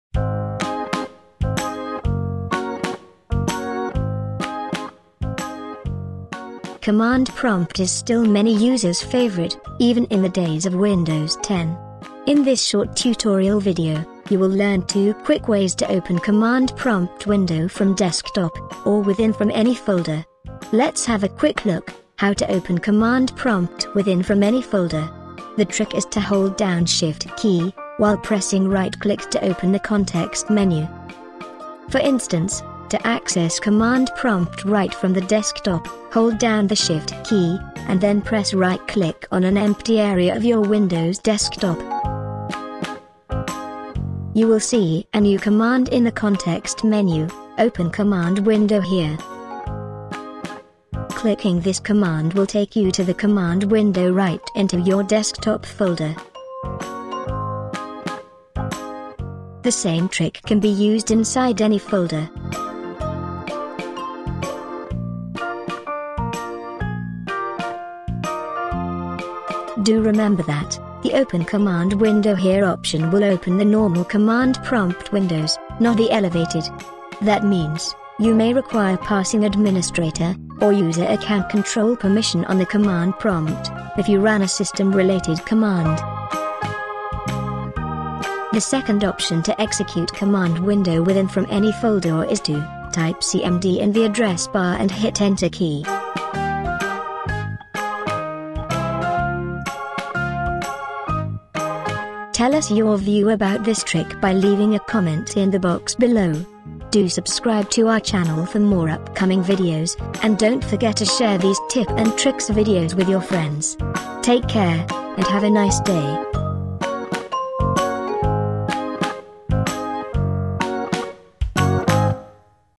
How to Open Commnad Prompt within Any Folder on Windows 10 | The Teacher
In this short Microsoft Windows 10 Tutorial, you will learn two easy ways to open Command Prompt right from your desktop or within any folder. Do remember that both tricks will open the normal command prompt, not in admin mode. So you may require administrator permission or pass the user account control if you run any system-related command.